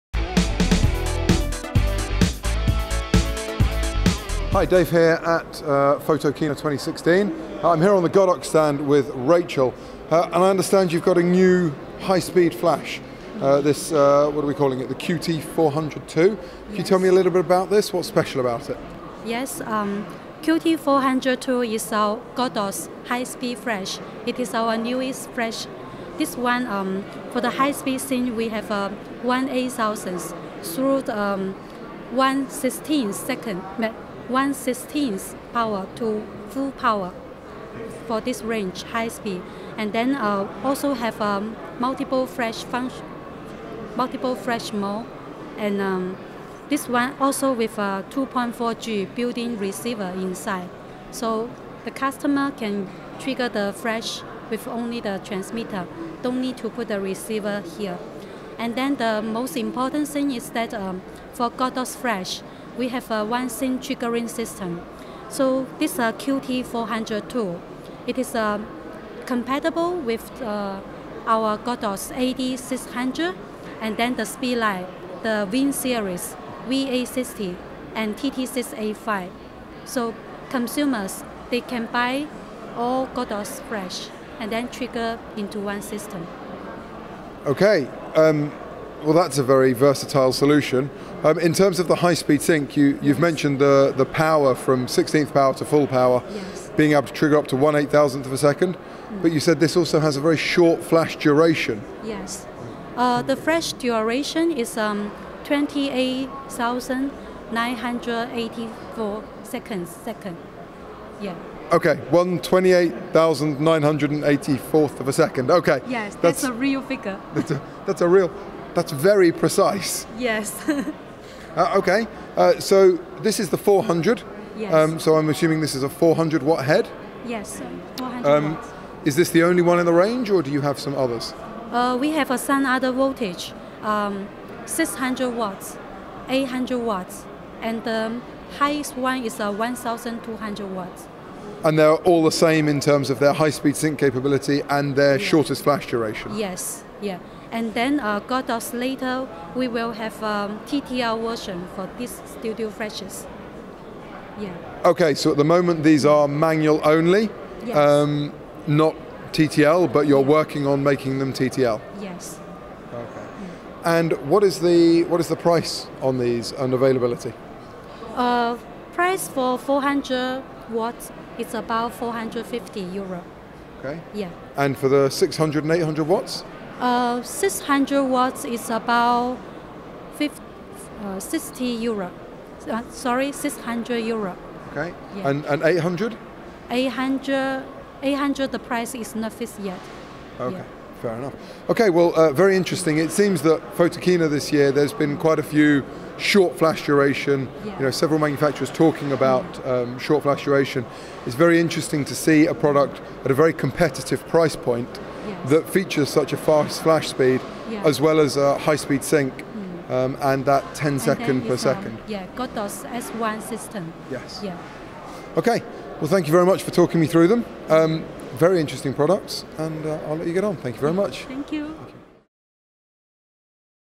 Photogearnews at Photokina 2016: Godox QT400 II High Speed Flash Head
Godox was showing their 1/8000 of a second high speed flash at Photokina 2016, their QT400 II, which has built in wireless, so you won't need to attach a receiver to the flash, you'll just need the transmitter.

There are a number of versions of this flash head, 400w, 600w, 800w, and 1200w, and there will eventually be a TTL version. The price for the 400w is €450, 600w is €600, and the 800w does not have a price yet.